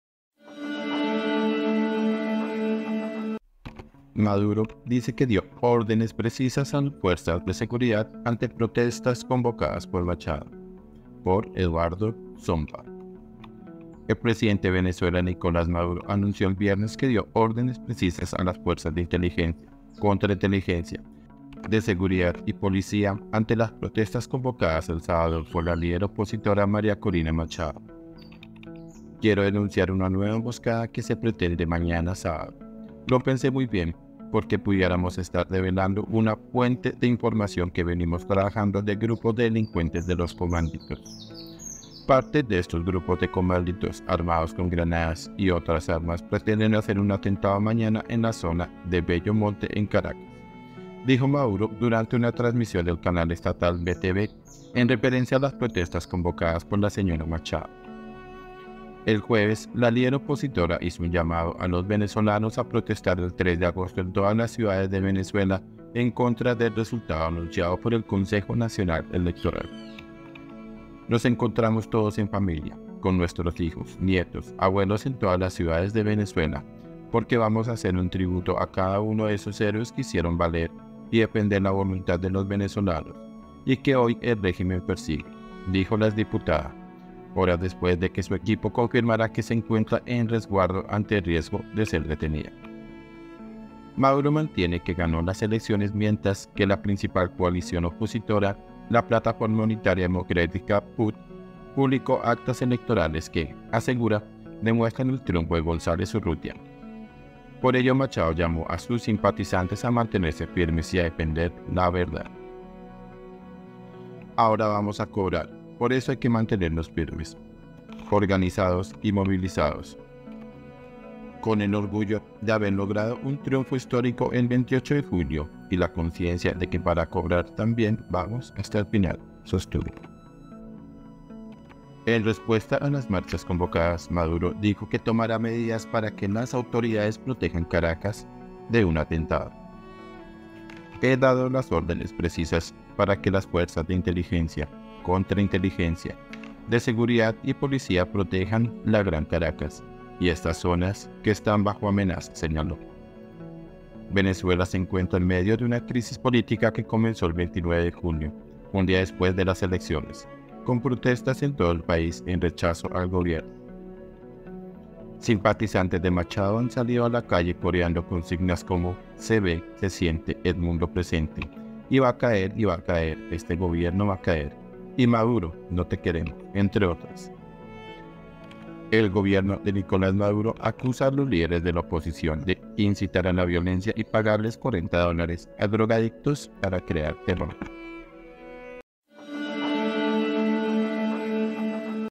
MADURO DICE QUE DIO ÓRDENES PRECISAS A FUERZAS DE SEGURIDAD ANTE PROTESTAS CONVOCADAS POR MACHADO
El presidente de Venezuela, Nicolás Maduro, anunció el viernes que dio «órdenes precisas» a las fuerzas de inteligencia, contrainteligencia, de seguridad y policía ante las protestas convocadas el sábado por la líder opositora, María Corina Machado.
The Epoch Times en español